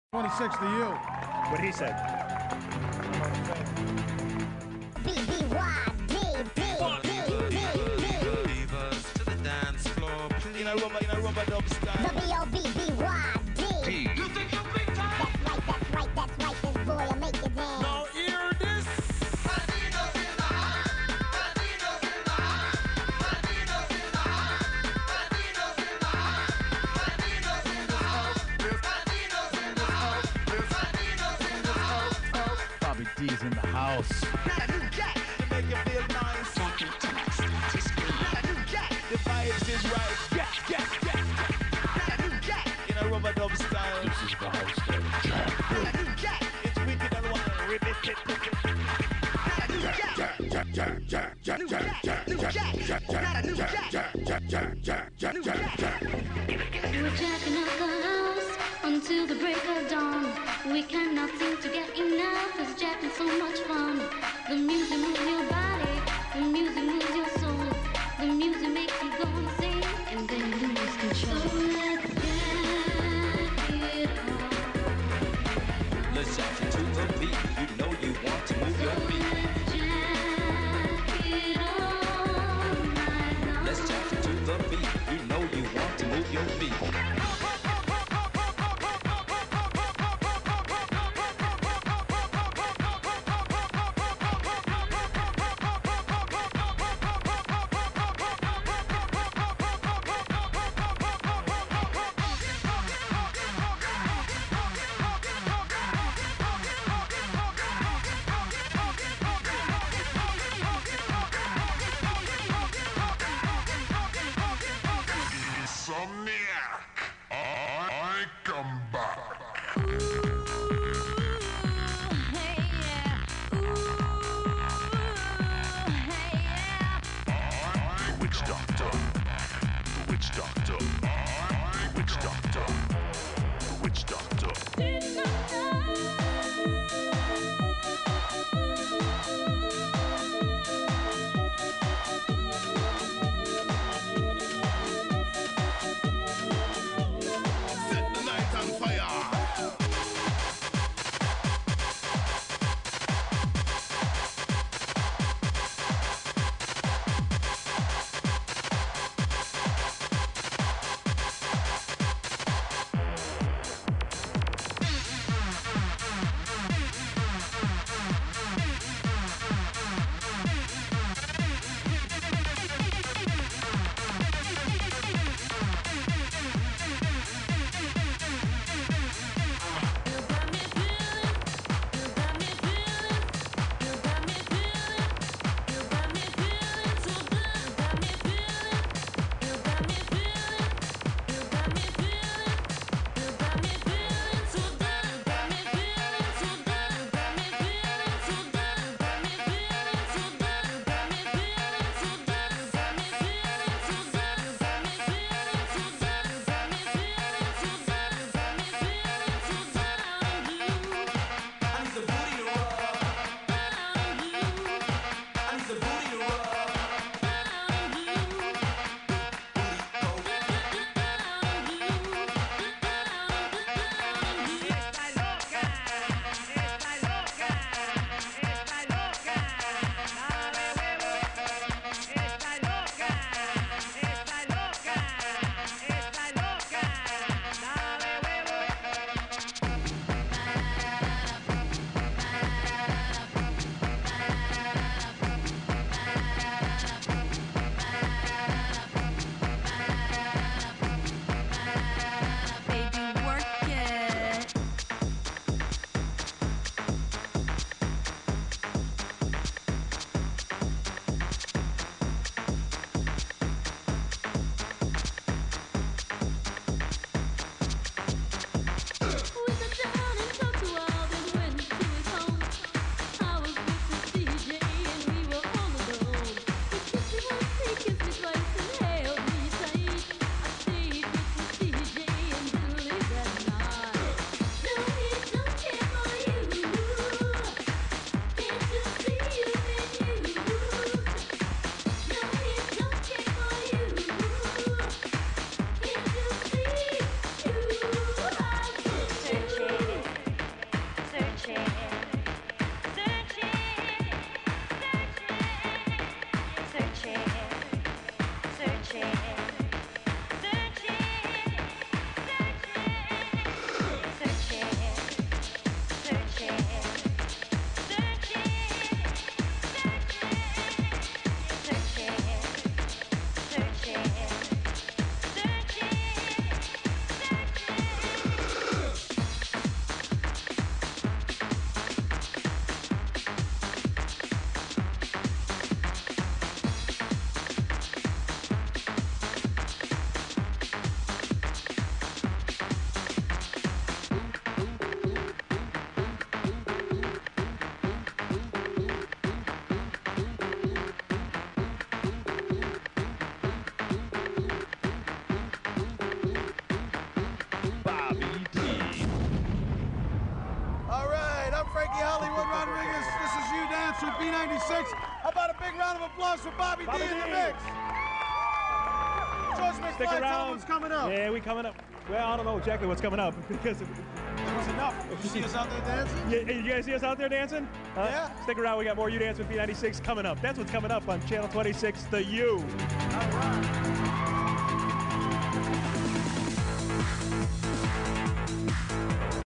BobbyD at U Dance with B96
This is one of the early on shows when the dance circle was getting alot of attention...